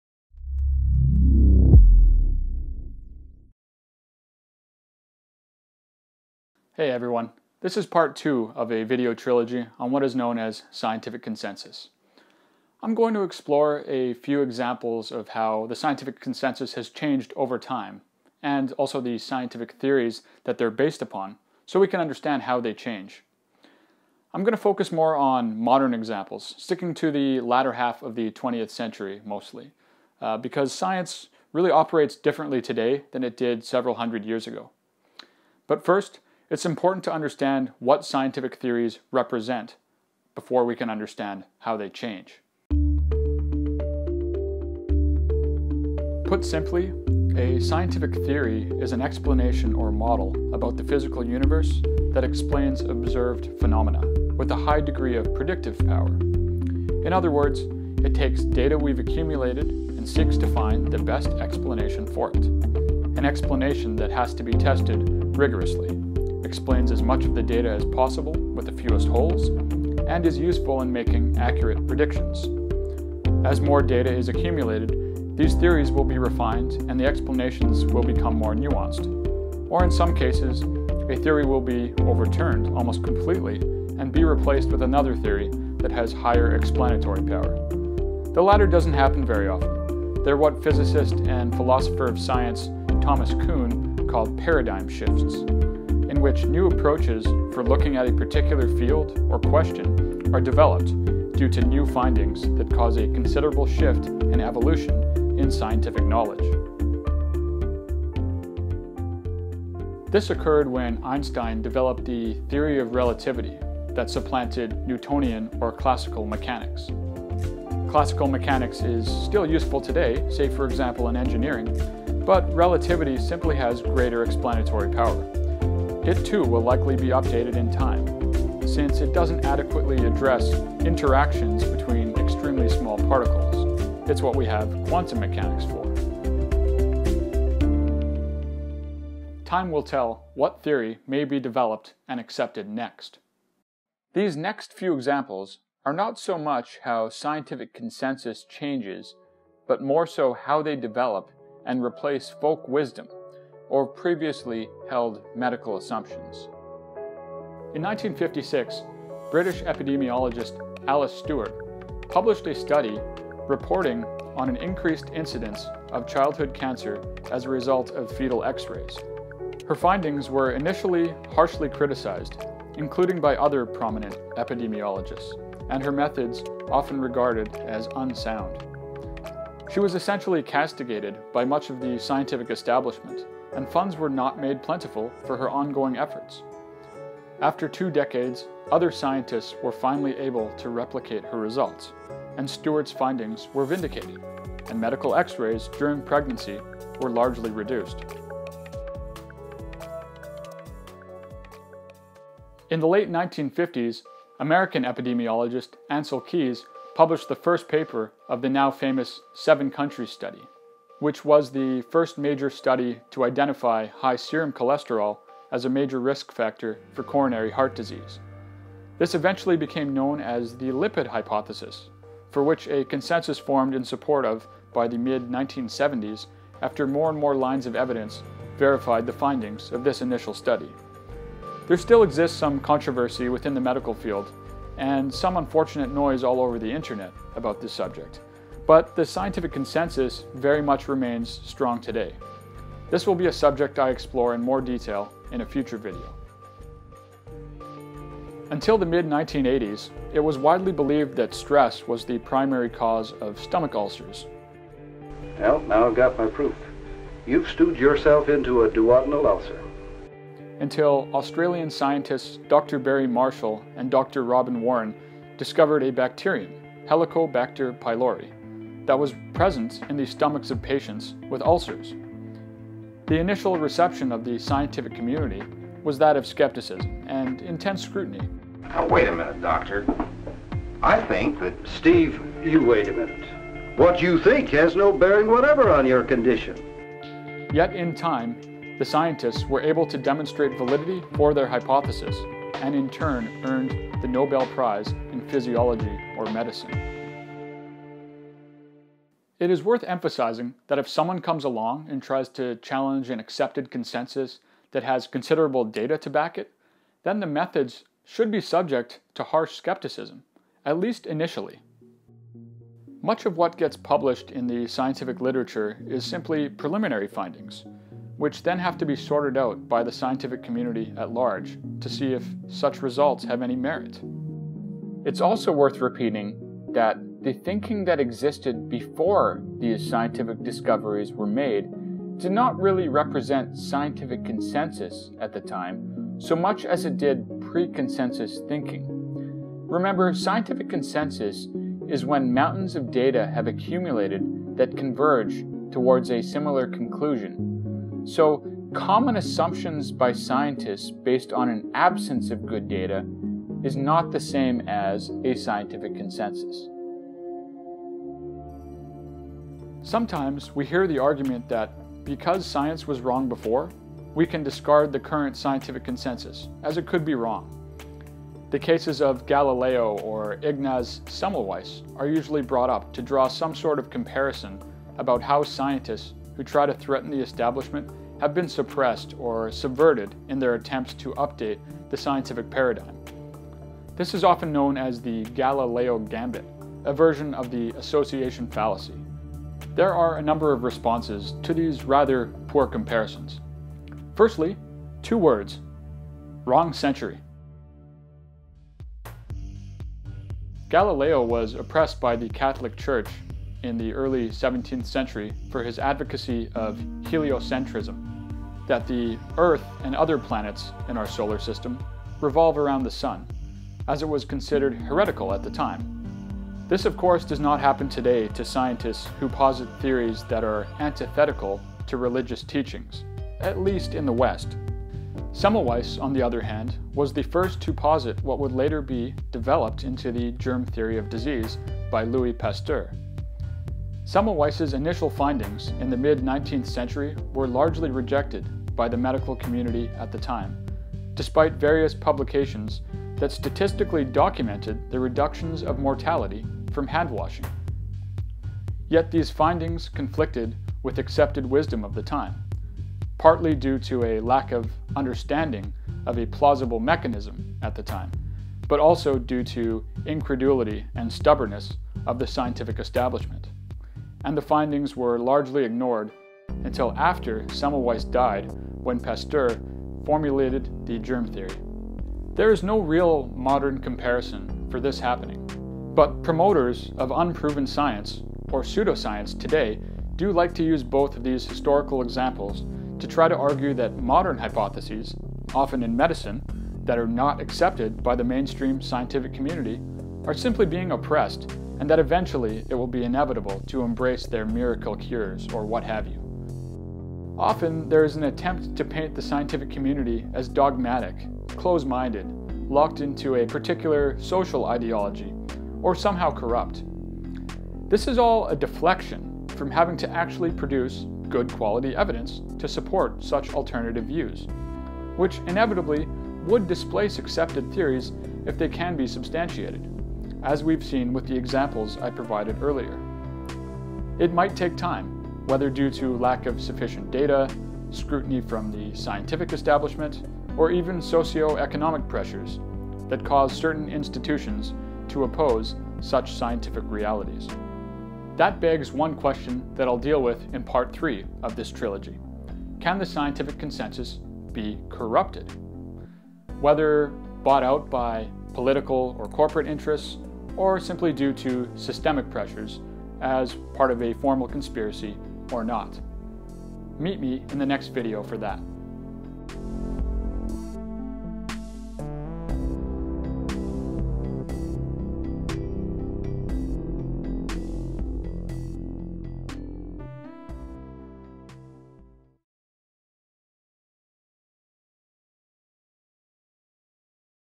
Science Literacy | 2.2 Scientific Consensus - How It Changes
(Part 2 of 3) How does scientific consensus change over time? What happens when new evidence emerges that challenges conventional scientific or folk wisdom? Does scientific consensus ever do a 180° flip? This video explores these questions and more.

SOURCES:

1:11 : “Science and Creationism: A View from the National Academy of Sciences, Second Edition.” 1999. P. 2. The National Academies Press.

“The Structure of Scientific Theories.” Stanford Encyclopedia of Philosophy. 5 Mar, 2015.

1:37 : Wikipedia. “Paradigm Shift”.

2:20 : “Even theories change”. Understanding Science: how science really works (Berkley).

3:16 : Stewart et al. “A Survey of Childhood Malignancies”. British Medical Journal. 28 Jun, 1958.

Stewart, A. & Kneale, G.W. “Radiation Dose Effects in Relation to Obstetric X-Rays and Childhood Cancers”. The Lancet. 6 Jun, 1970.

3:54 : Steinberg, D. “Thematic review series: The Pathogenesis of Atherosclerosis. An interpretive history of the cholesterol controversy: part I”. Journal of Lipid Research. 21 Apr, 2004.

4:54 : Schulz, Kathryn. “Stress Doesn't Cause Ulcers! Or, How To Win a Nobel Prize in One Easy Lesson: Barry Marshall on Being ... Right”. Slate. 9 Sept, 2010.

6:26 : Wikipedia. “Association fallacy: Galileo Gambit”